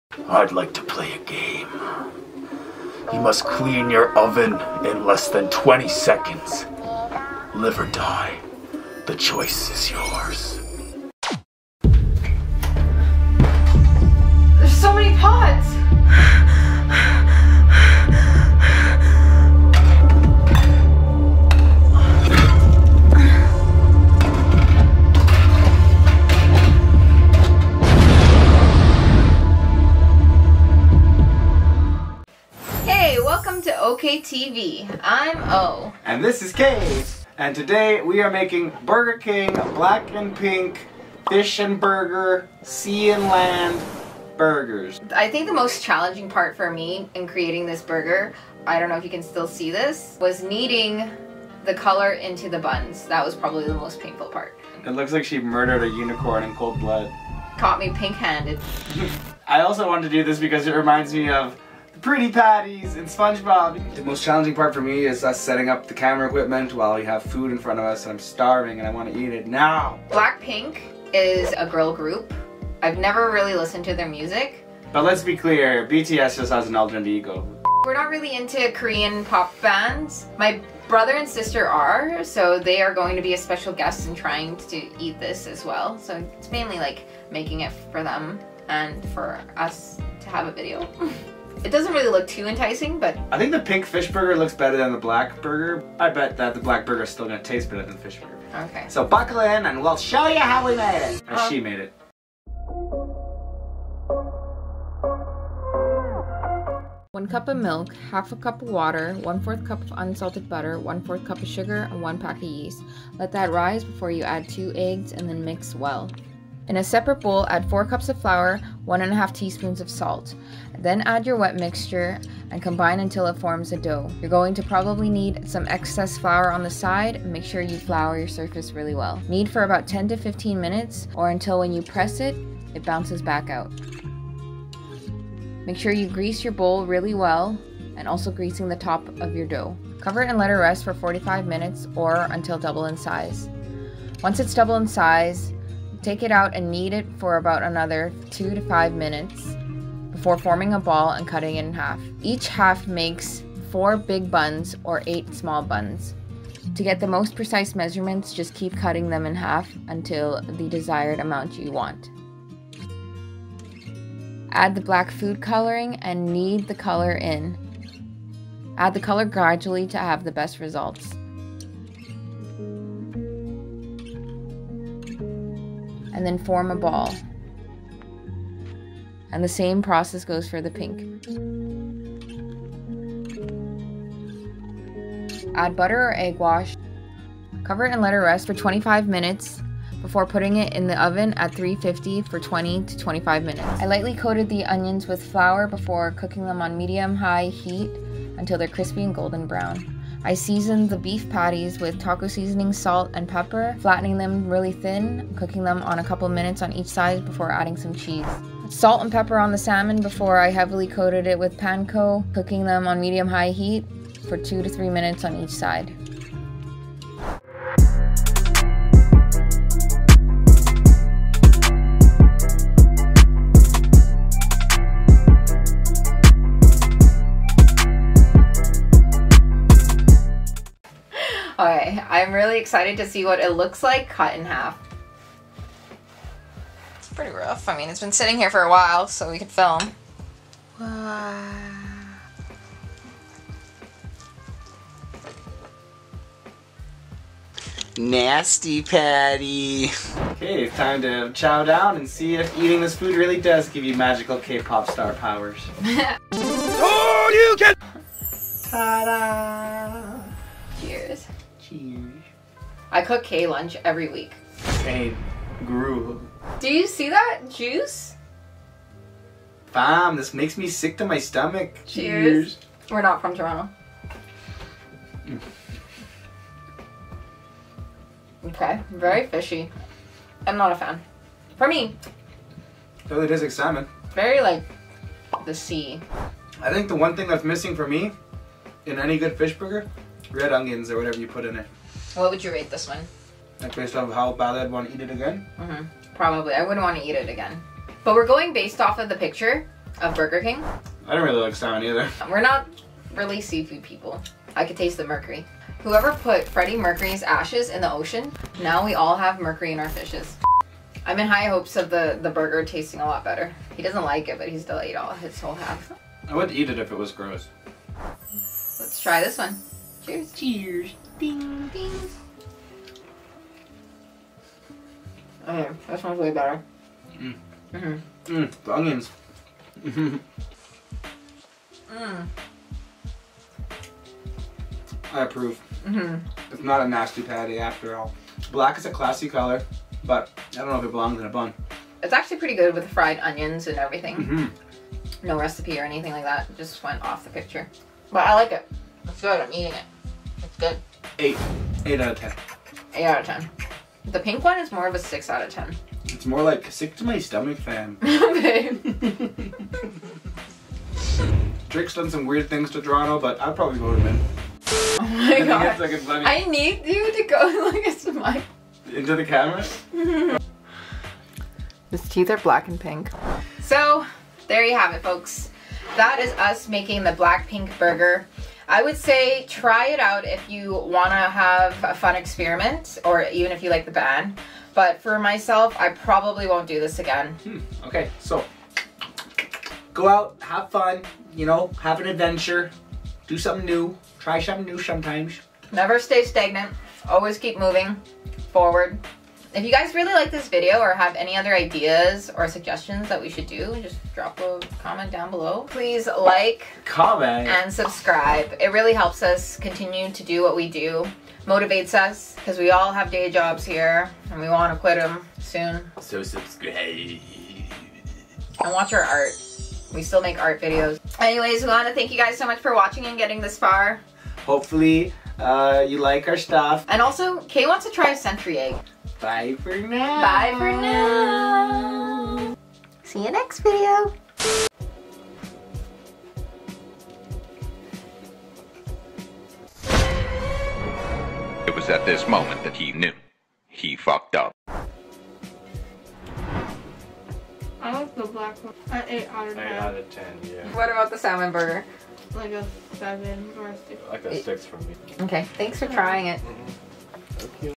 BLACKPINK BURGER (Burger King Thailand) : HOW TO MAKE
This was a challenge to say the least. It was very fun, and I always love sharing our experiences on the inter-webs. Even though I wouldn't make them again, I recommend trying it out, just please learn from my mistake and wear cooking gloves! Hope you guys enjoyed this video, see you again in a couple of weeks! - Oh

BUN RECIPE (tasty) Serving for 8 large buns or 16 small buns:
1 cup whole milk(240 mL)
½ cup warm water(120 mL)
¼ cup unsalted butter(60 mL), melted
¼ cup sugar(50 g)
2 ¼ teaspoons active dry yeast, 1 packet
4 cups all-purpose flour(500 g), plus more for dusting
1 ½ teaspoons kosher salt
2 large eggs, room temperature
neutral oil, for greasing
sea salt, for sprinkling

BEEF BURGER RECIPE:
Lean Ground Beef (454 g) can make up to 8 smash patties
White cheddar or mozzarella
Salt and pepper to taste
1/2 a pack of taco seasoning
Hot mustard to taste
Ketchup to taste

SALMON BURGER RECIPE:
1 salmon fillet (0.303 g) cut into 2 pieces
1 cup of panko bread crumbs
Tzatziki sauce to taste

Awesome music we used!